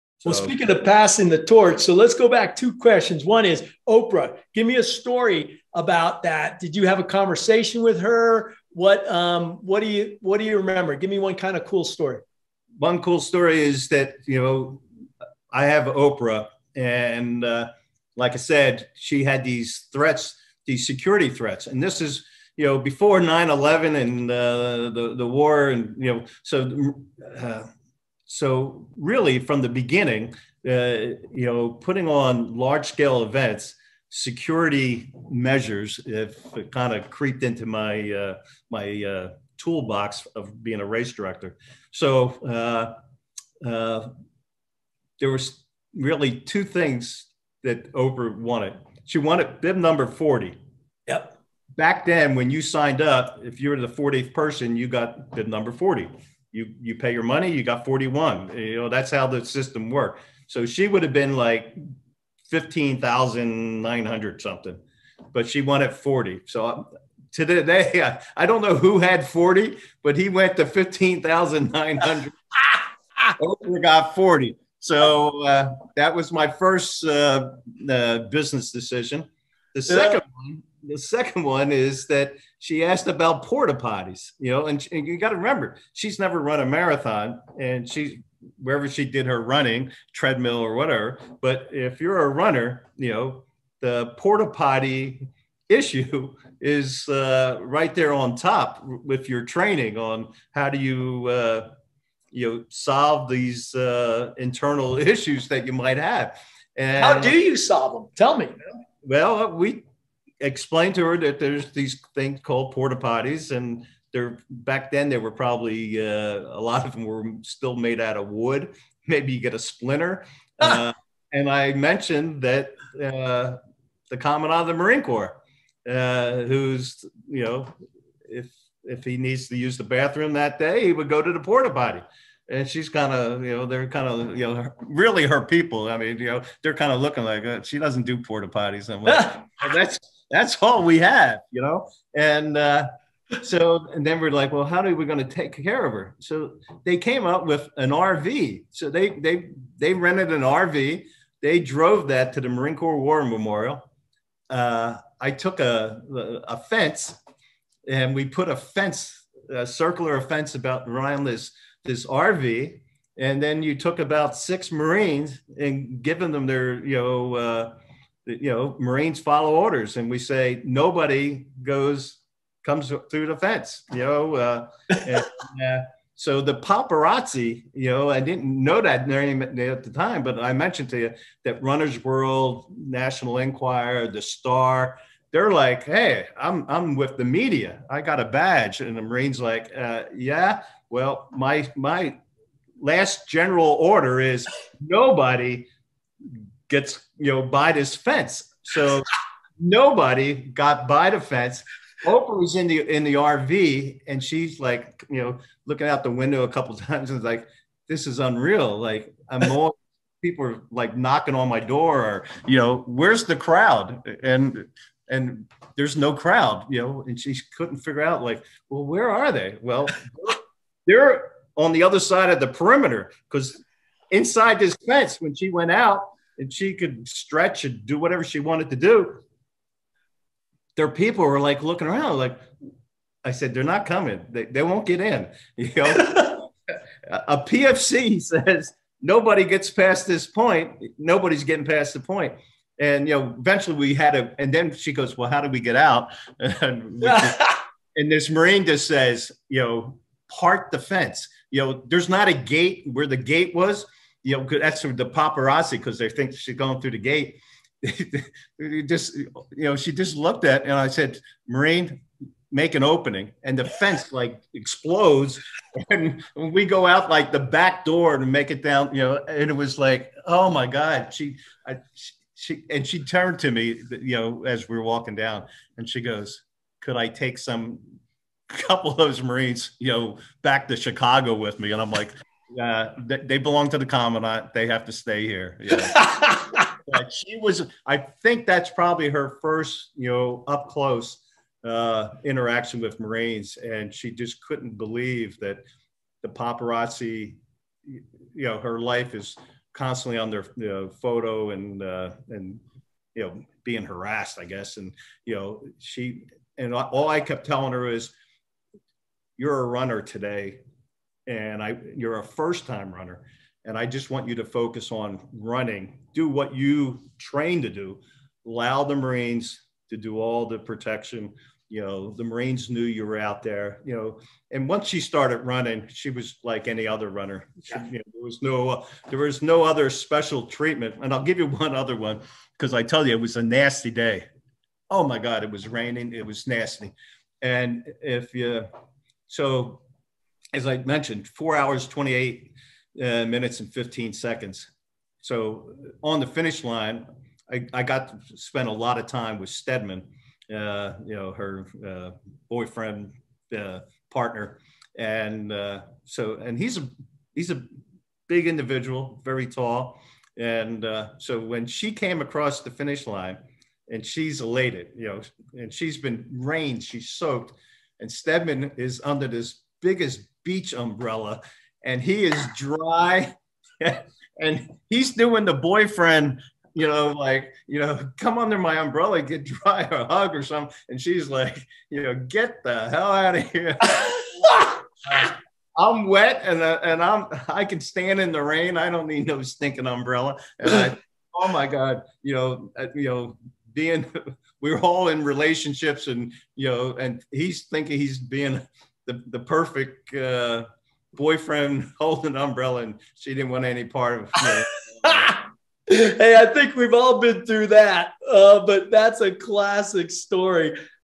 Rick Nealis: Behind the Scenes Stories From Oprah Winfrey Running the Marine Corps Marathon
Rick Nealis has been the Race Director of the Marine Corps Marathon for nearly thirty years. He’s a former Marine and a people person who loves the camaraderie that comes with organizing and being part of such a large event. Rick is also an accomplished runner whose lifelong mission is to encourage more people to take up running to improve their physical and mental health.

Topics Discussed:
- The legacy of Major Megan McClung
- The history of the Marine Corps Marathon
- Guarding the Olympic torch
- Famous participants
- Rick’s motivational message